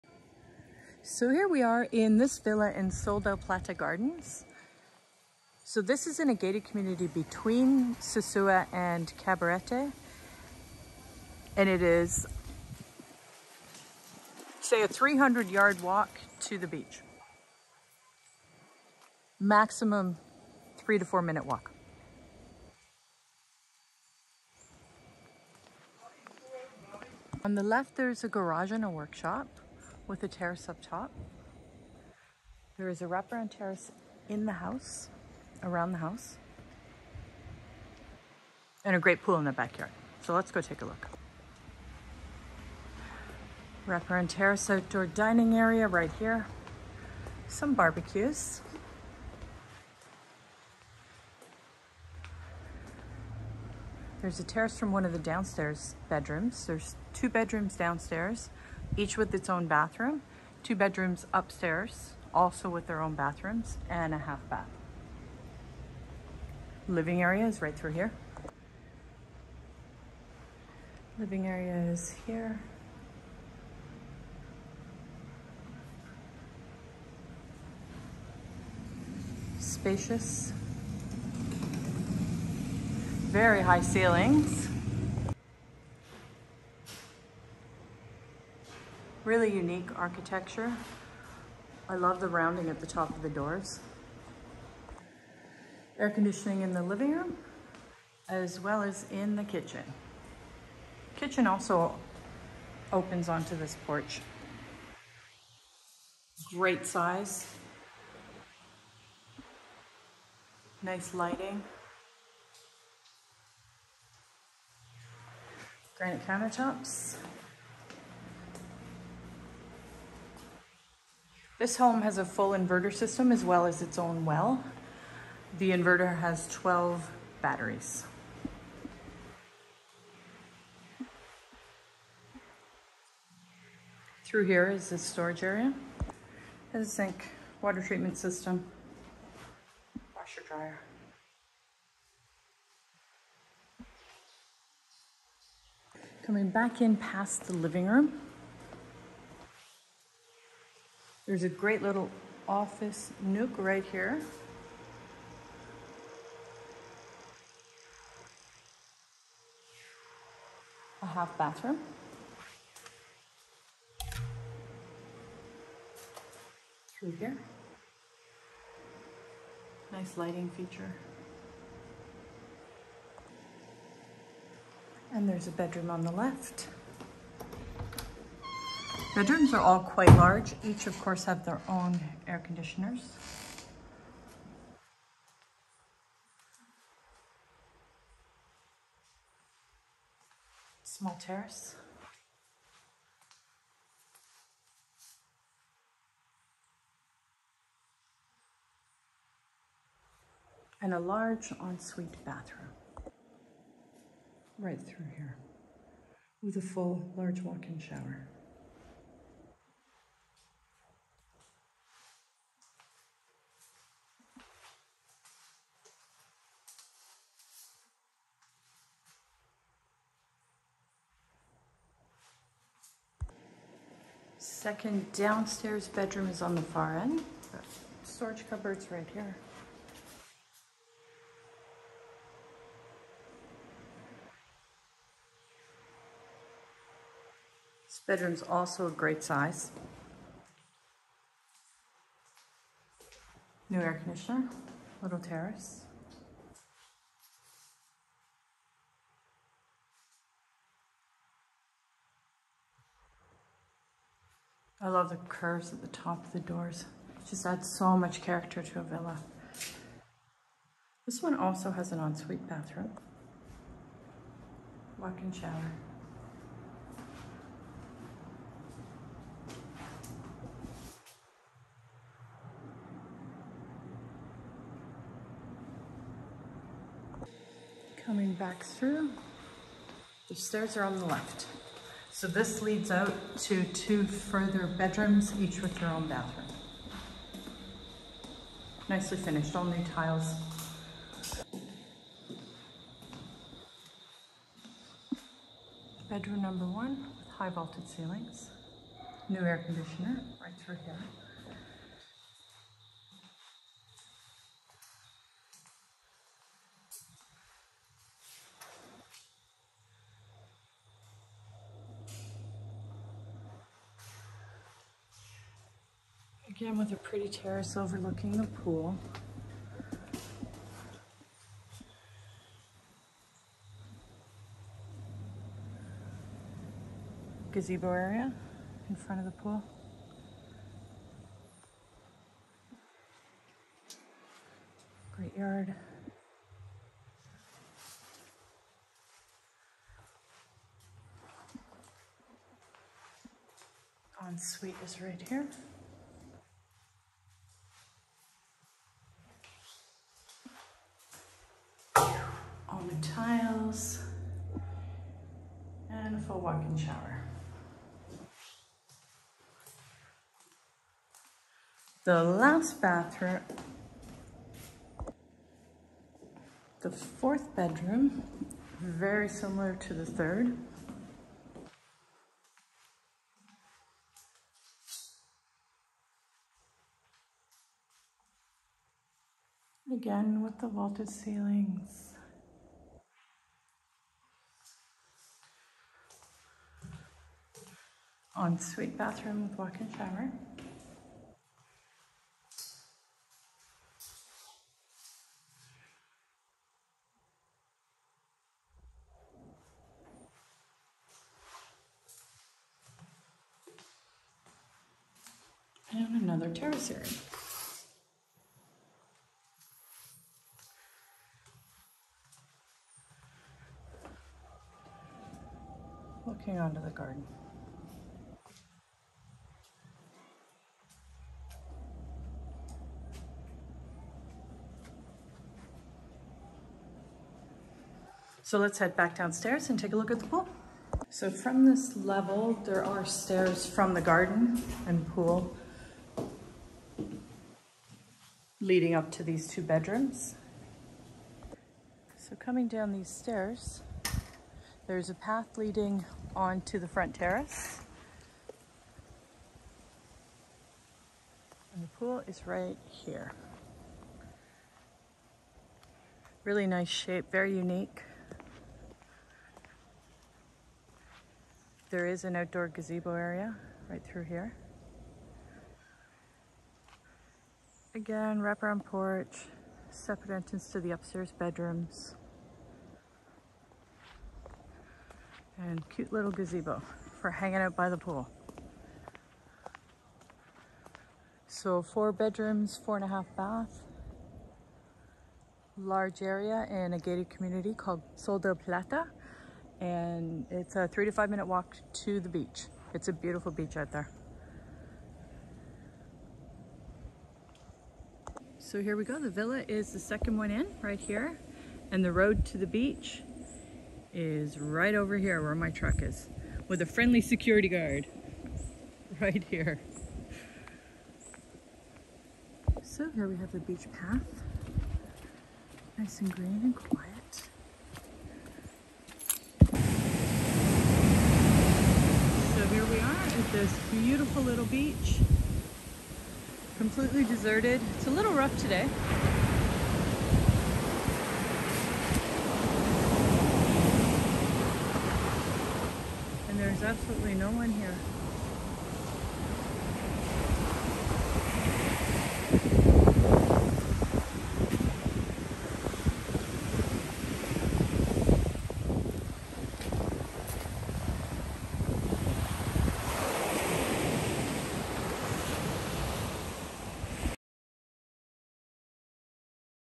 4 Bedroom House in Cabarete - Steps to Beach $595,000
Spacious 4 bedroom 4.5 bathroom villa is located in Sol del Plata Gardens a gated community between Sosua and Cabarete in the Dominican Republic only steps to the beach.

On the main level is a large outdoor terrace, a spacious living room with vaulted ceilings and an large kitchen with a fantastic island. 2 bedrooms each with an ensuite bathroom. A large laundry room and a half bathroom.

Upstairs are 2 more bedrooms and bathrooms.

Each bedroom has a split unit air conditioner plus one in the living room and one in the kitchen.  A well on the lot ensures water 24/7 also a 12 battery inverter system provides back up power.

Great pool and outdoor sitting area in the yard to allow the owners to enjoy their outdoor tropical lifestyle. This is truly a beautiful home only a few minutes from the beach.

Find more fantastic Sosua and Cabarete real estate listings on our website here

Full listing here